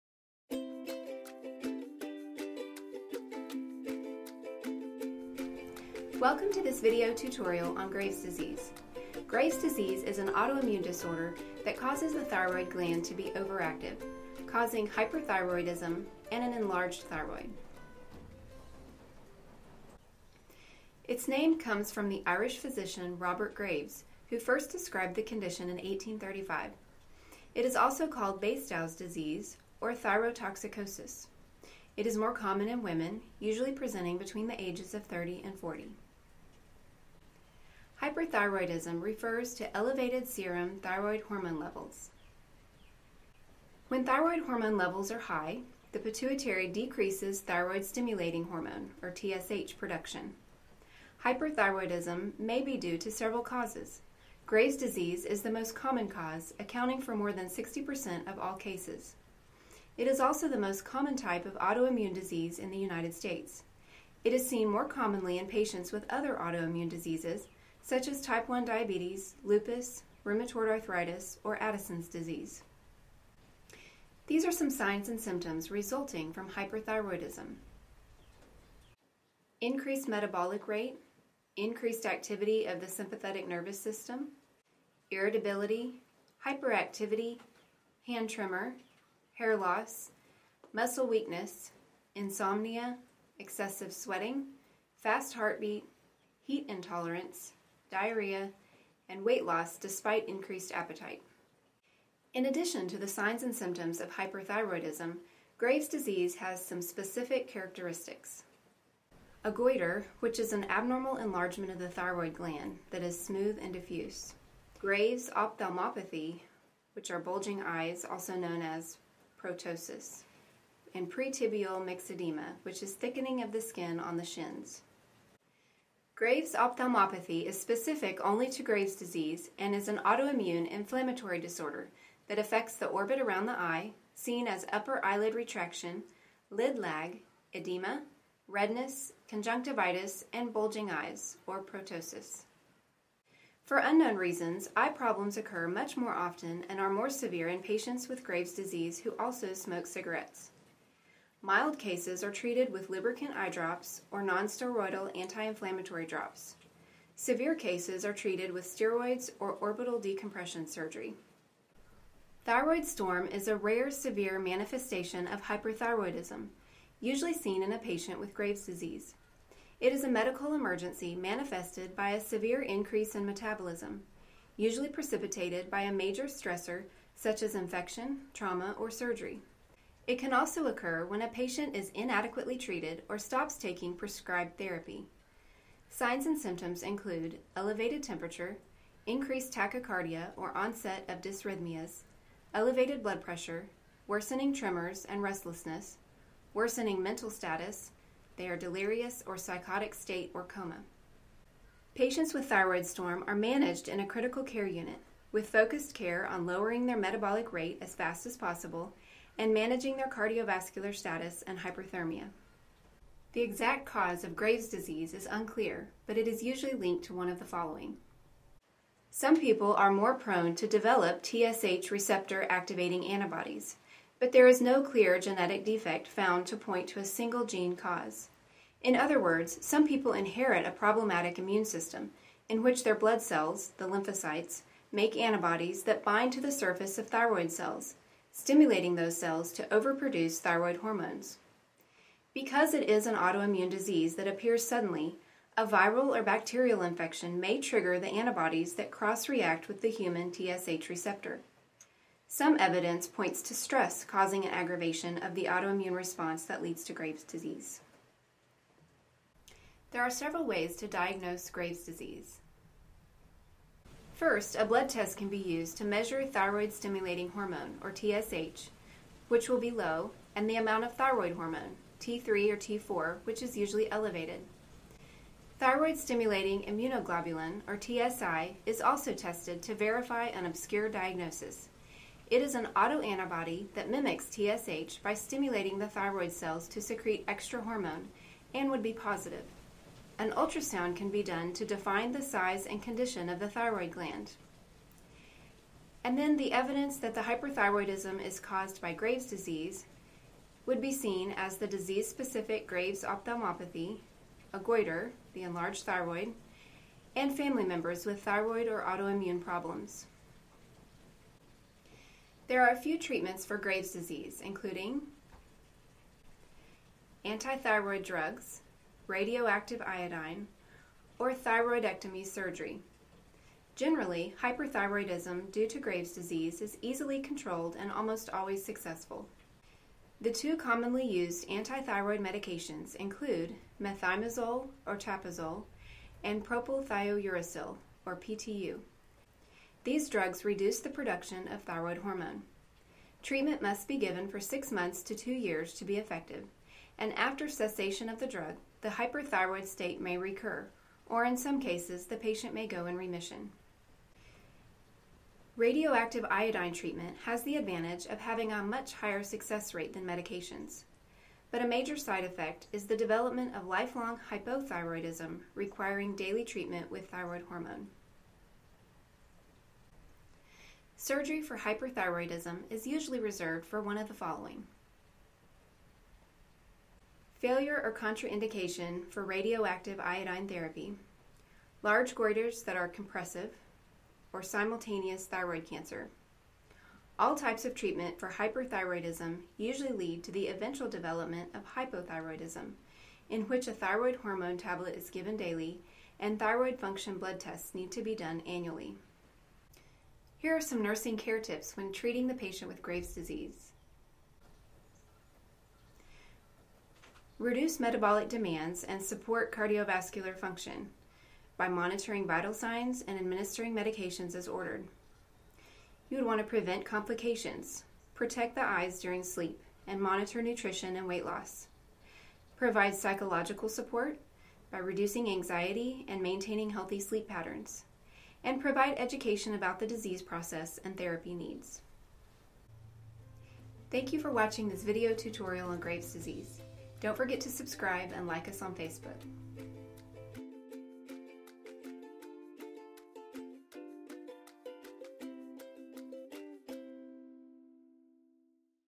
NCLEX RN Review - Graves Disease
Graves’ disease is an autoimmune disorder that causes the thyroid gland to be overactive – causing hyperthyroidism & an enlarged thyroid.